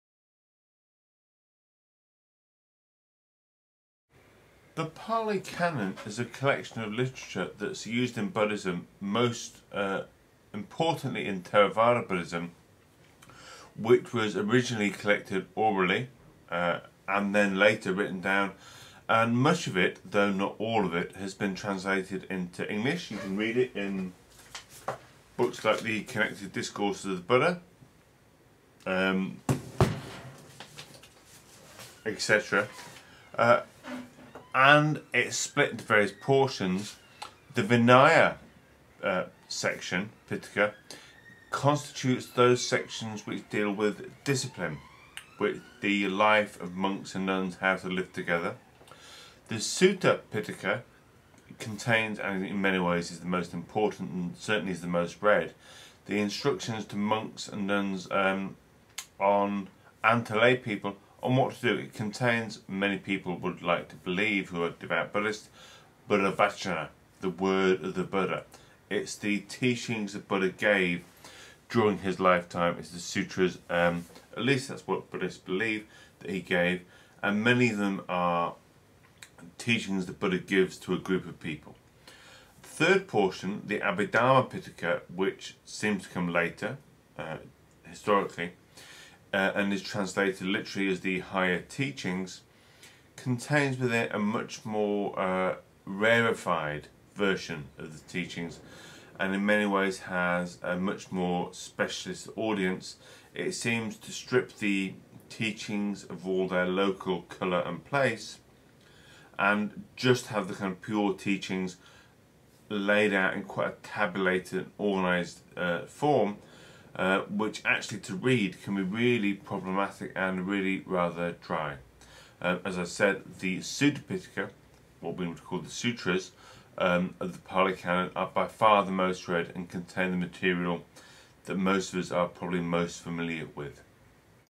Pali Canon
David Webster introduces the Pali Canon